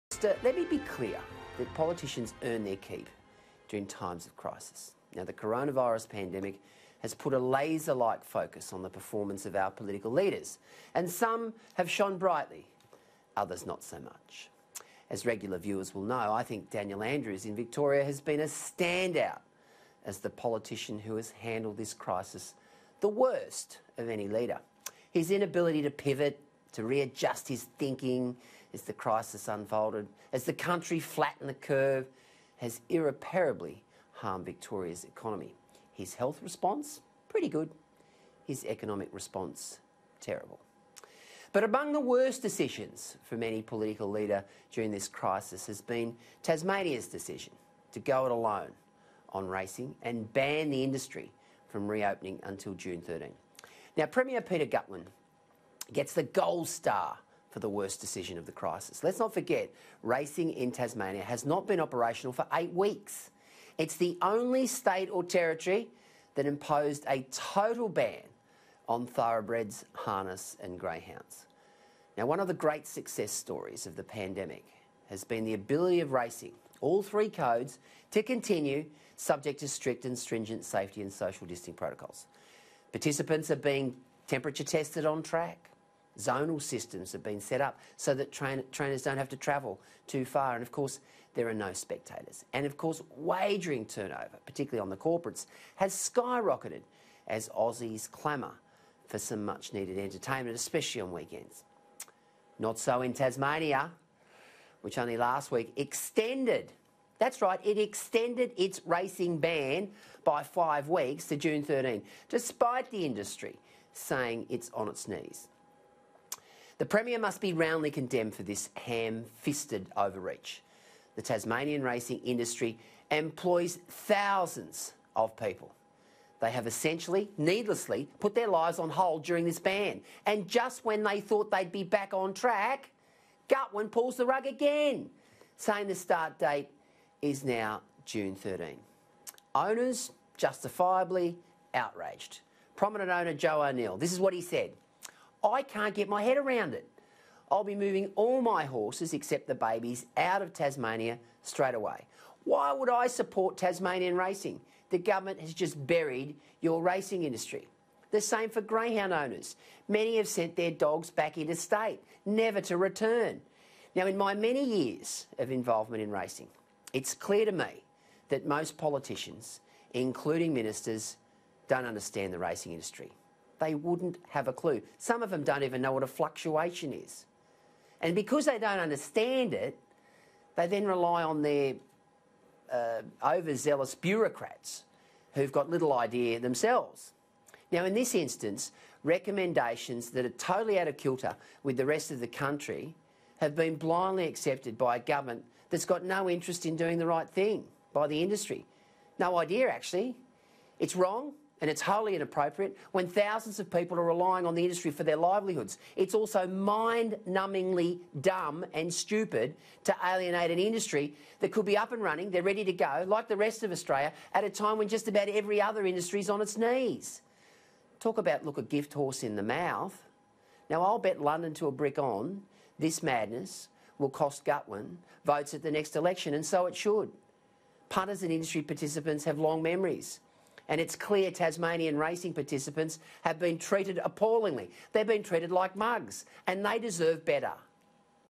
Tasmanian Premier wins ‘gold star’ for worst COVID-19 decision
Sky News host Peter Gleeson says Tasmanian Premier Peter Gutwein must be “roundly condemned for this ham fisted over reach” as the Premier extends the ban on all racing in the state for an additional five weeks.

While many industries across the country have ground to a halt, racing in most states has remained, but in Tasmania it has “not been operational for eight weeks”.

Mr Gutwein announced last week the ban will be extended until June 13 despite Tasmania’s racing industry “saying it’s on its knees”.

Mr Gleeson said although Victorian Premier Daniel Andrews is a standout as the politician who has “handled this crisis the worst of any leader,” Premier Gutwein wins the “gold star for worst decision of the crisis”.

“The Tasmanian racing industry employs thousands of people; they have essentially needlessly put their lives on hold during this ban,” he said.

“I will bet London to a brick on this madness costing Gutwein votes at the next election; punters and industry participants have long memories”.